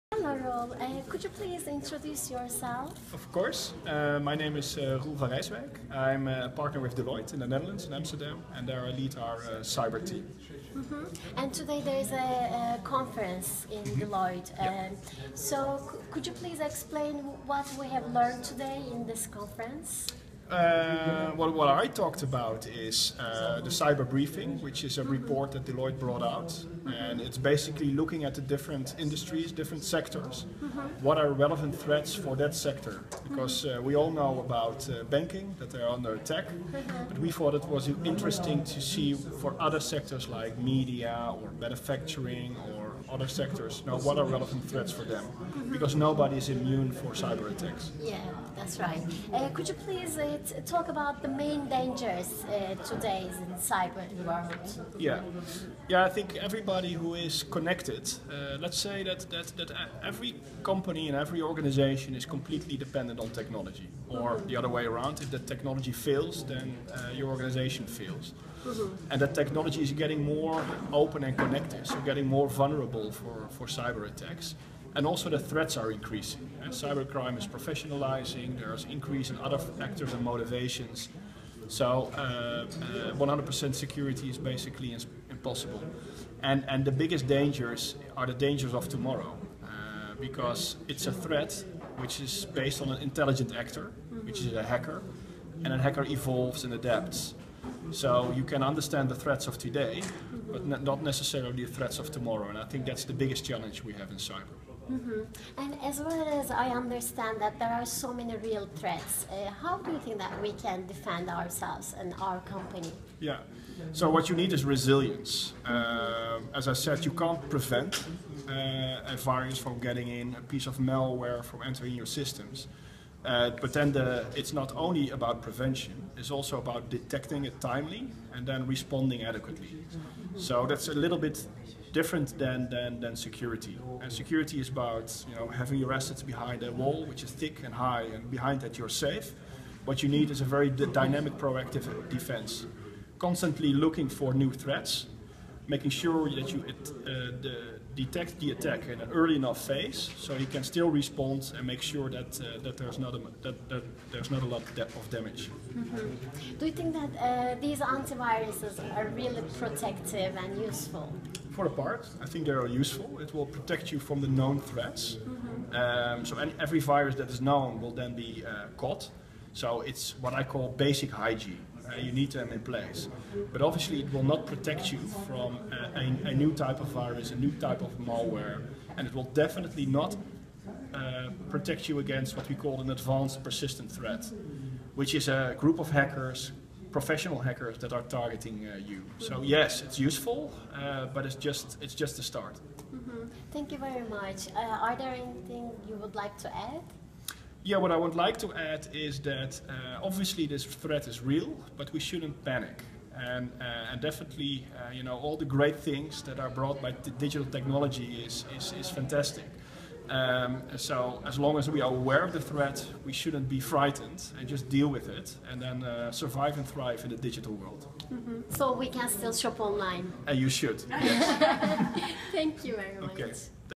Roel Van Rijsewijk - Deloitte Siber Güvenlik Etkinliği
Siber suçların artık devlet destekli casusluk veya yalnızlığı seven bir bilgisayar korsanının yaptıklarıyla sınırlı olmadığı günümüzde, Deloitte Türkiye, şirketlerin siber saldırılarla nasıl başa çıkması gerektiği üzerine eğilen bir toplantı gerçekleştirdi.

Bu toplantı le ilgili detayı linkinden okuyabilirsiniz.